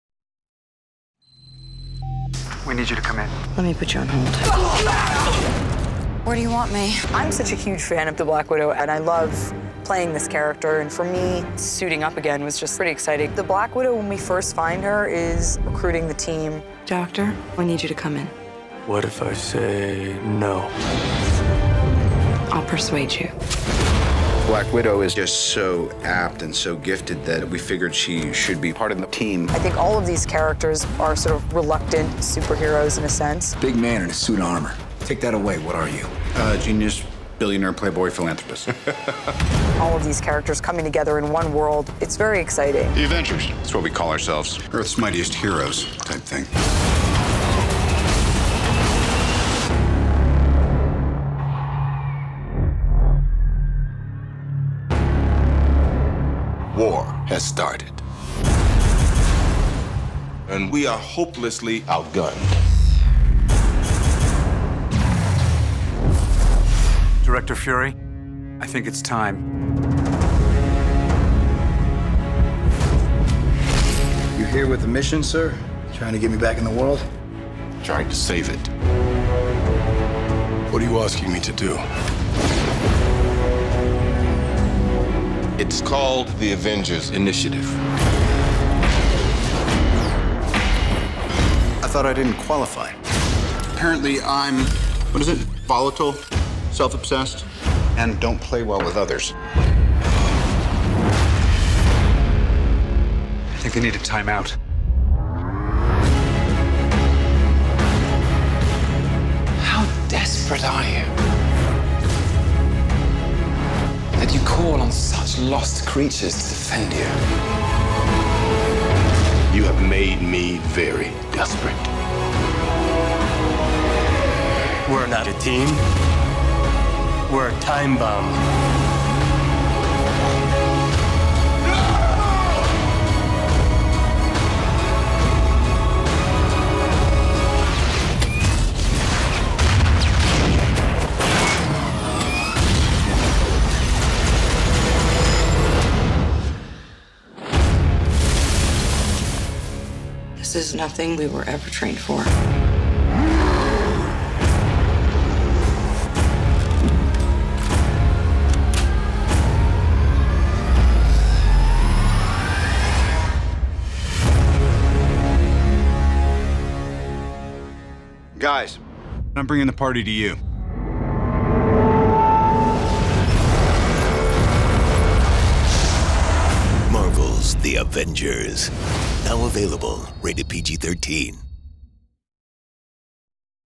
The Black Widow
Scarlett Johansonn talks about what it's like to be The Black Widow, one of the world's
greatest spies and quite possibly the world's most skilled assassin!